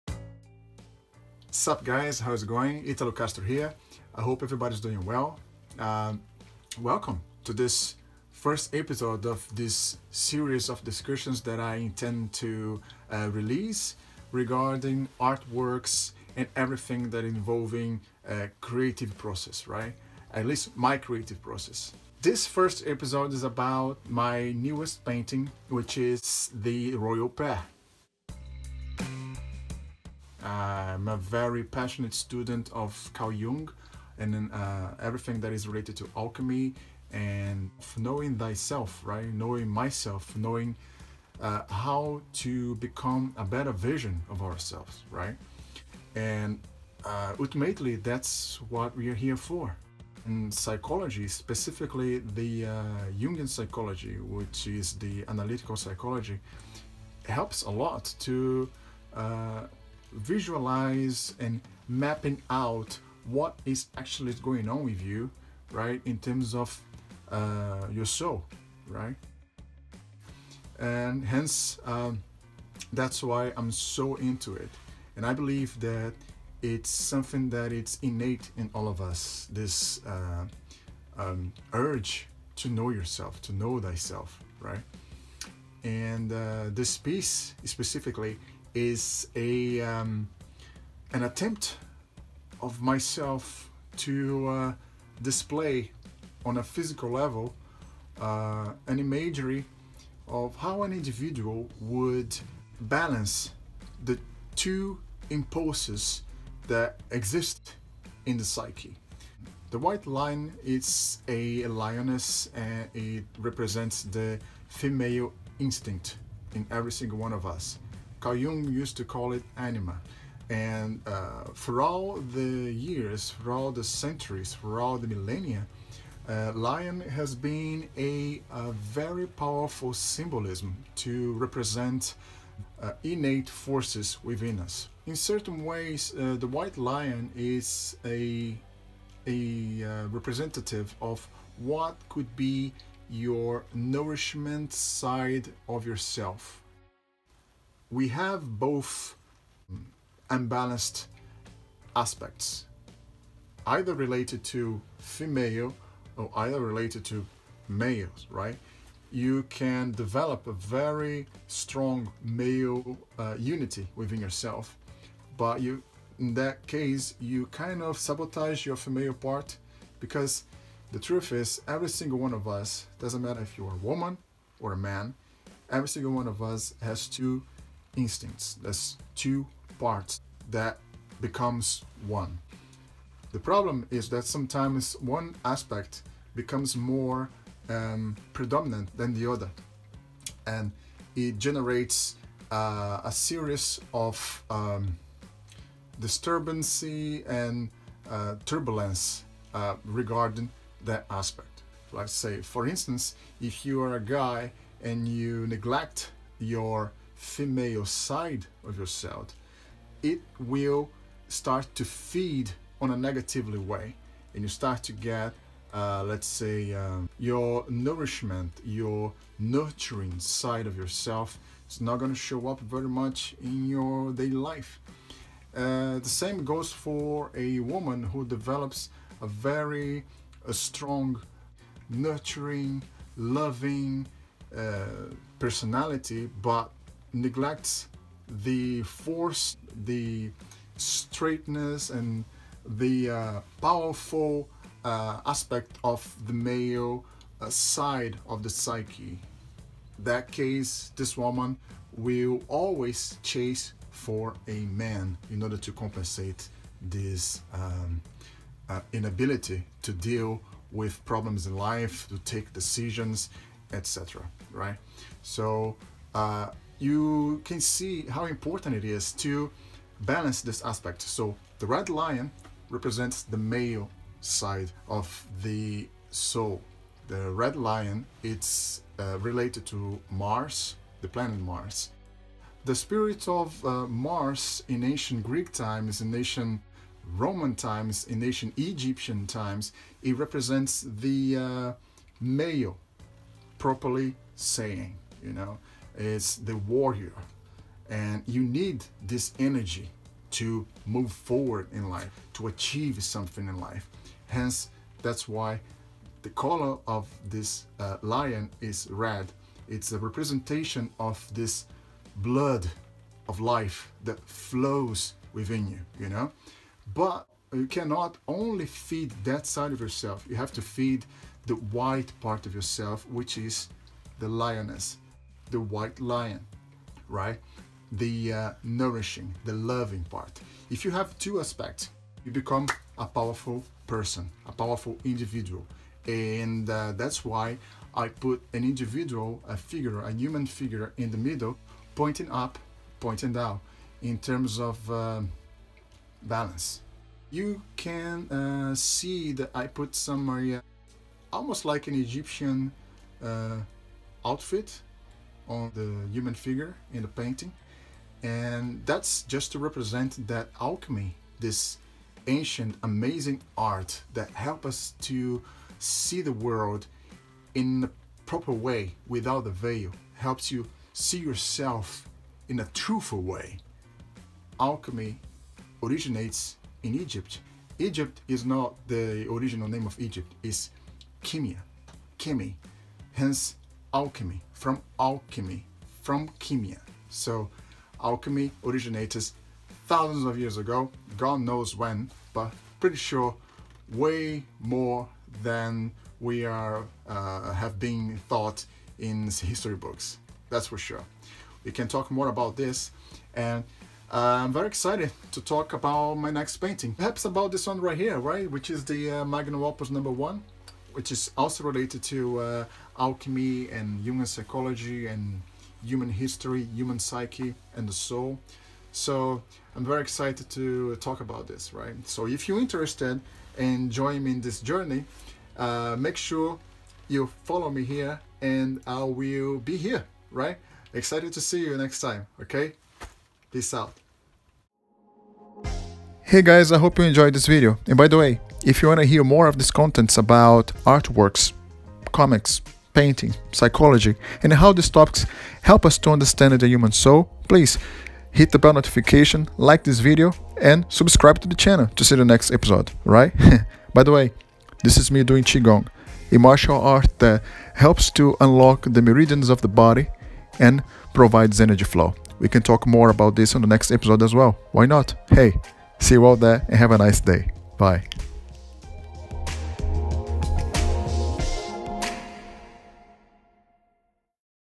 Mastering Thyself, Art Analysis And Overcoming Anxiety
Hey everyone!
My name is Italo Castro. I'm an actor, fine artist, comic book creator and designer.
This is the first video of my Youtube channel, a place where I will be showing and discussing my artworks such as paintings, comics and creative process. The intention is to try to understand the aspects of our soul and how the integration of ancient archetypes could help us improve our mental, physical and spiritual health.

In this  episode I talked about my most recent painting up to date called "The Royal Pair", as well as the reason why I decided to portray this ancient alchemical allegory of the lion vs lioness representing the battle of these forces within us.